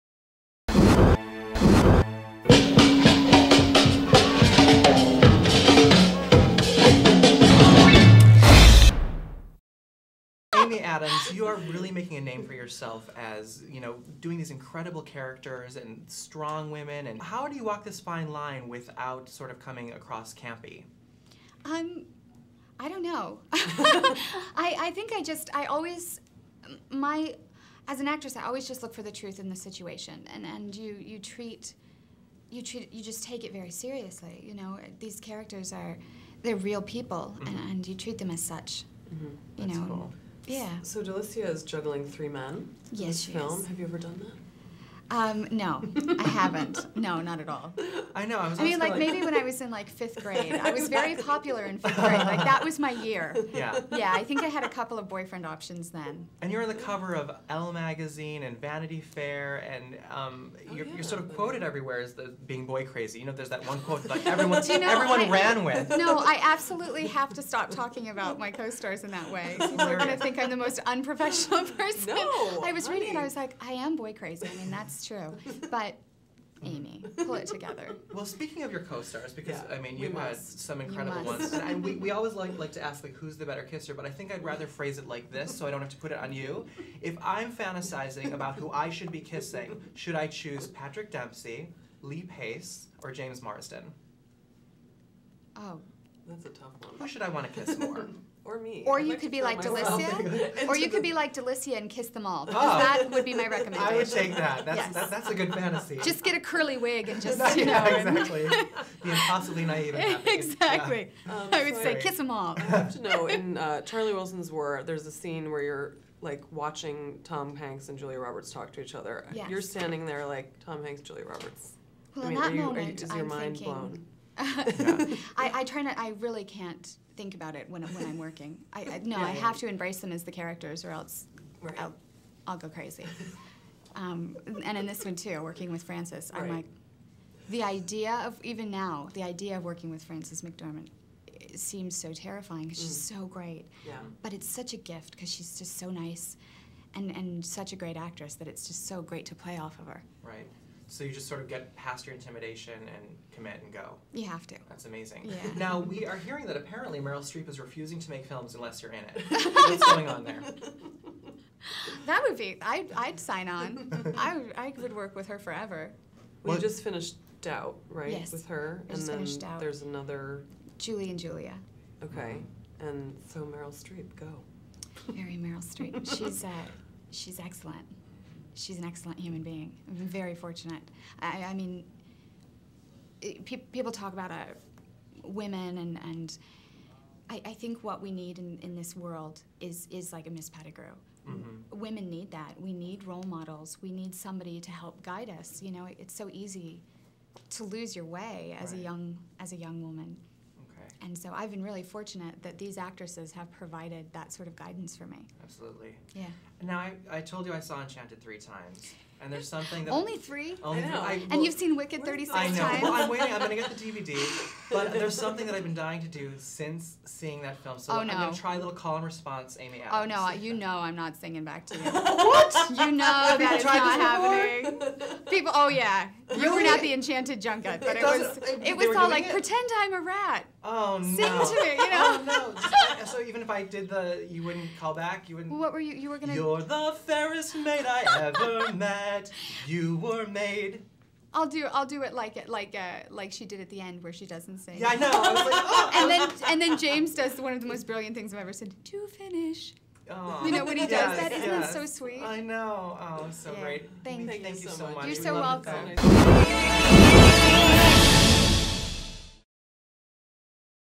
Pride Radio AA interview
Ryan & Caroline talk to Amy Adams about her movie "Miss Pettigrew Lives for a Day."